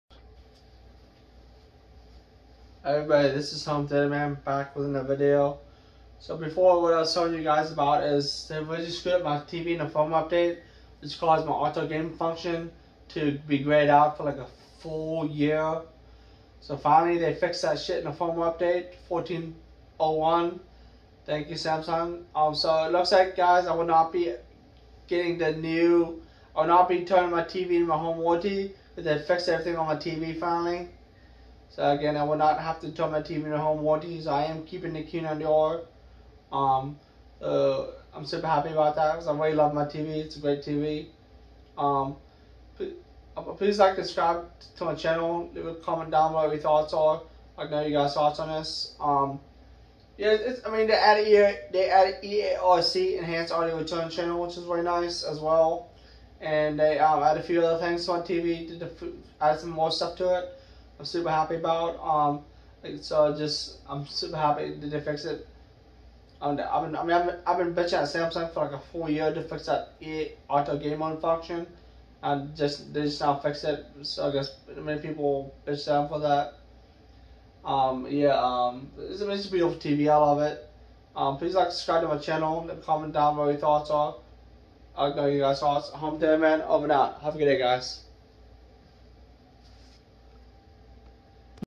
Samsung Finally Fixed the Auto Game Mode Function on All Samsung Q90R TV With Firmware Update 1401
Auto Game Mode is Now Working again  thru Firmware 1401